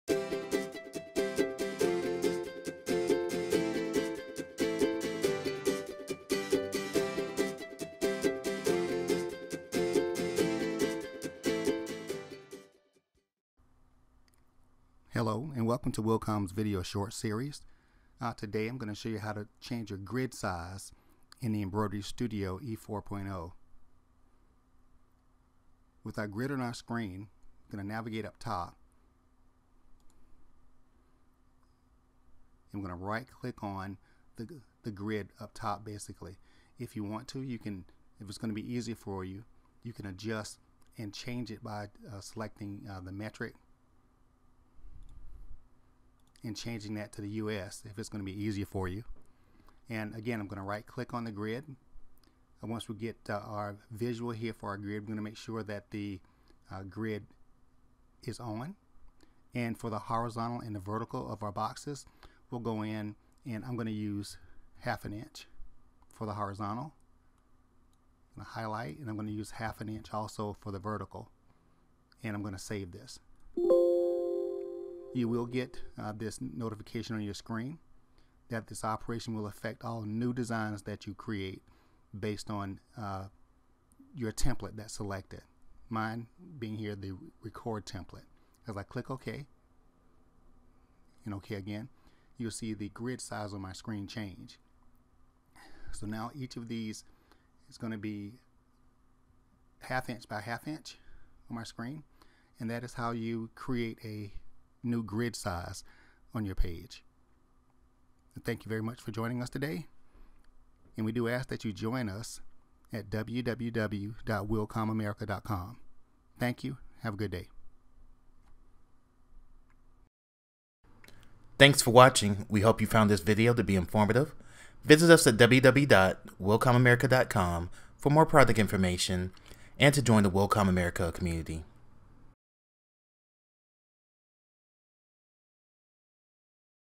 How To Change The Grid Size in EmbroideryStudio e4
Short video explaining how to change the grid size of a template in EmbroideryStudio e4